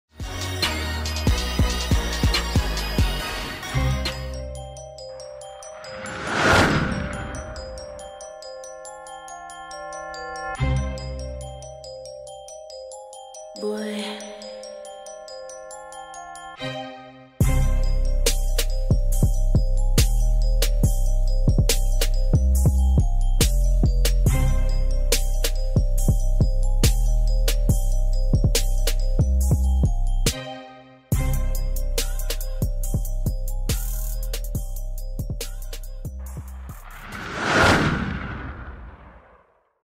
Daniel Cutting / Freestylers FC Lineup Animation | PeakGraphicz x CP7Football
Here is the animation that Curtis & I put together for Daniel Cutting for the Freestylers FC match against Hashtag United, captained by Spencer FC. Hope you enjoy, let me know what you think!

Special Thanks to CP7 Football for his help with the animation side of things, appreciate it mate!